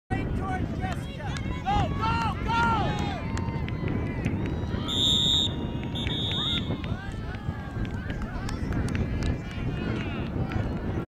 What if it went in?
Ref blows whistle to end game w/ball in mid air